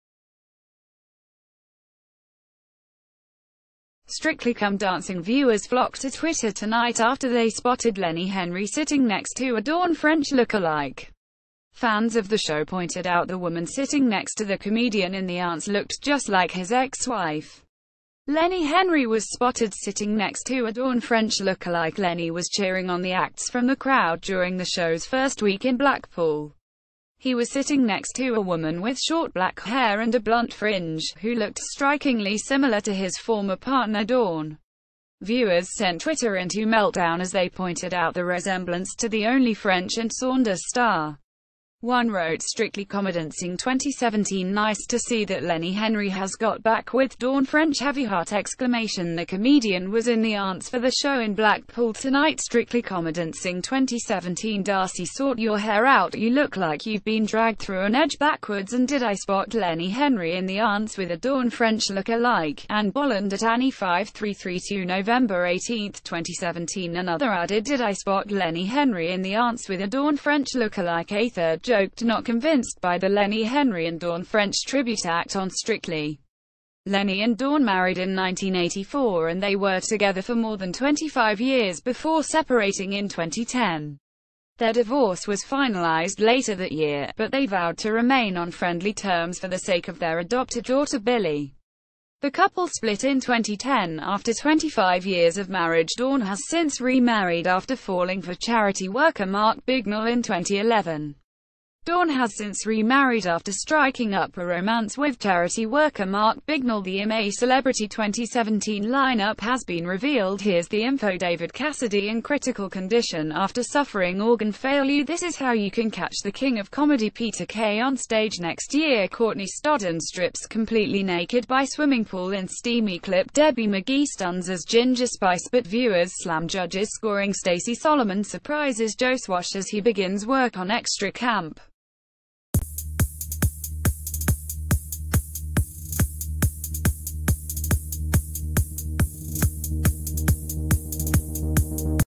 Strictly viewers spot lenny henry sitting next to a 'dawn french lookalike'| UK News 24H
Strictly viewers spot lenny henry sitting next to a 'dawn french lookalike'
STRICTLY Come Dancing viewers flocked to Twitter tonight after they spotted Lenny Henry sitting next to a Dawn French lookalike. Fans of the show poin...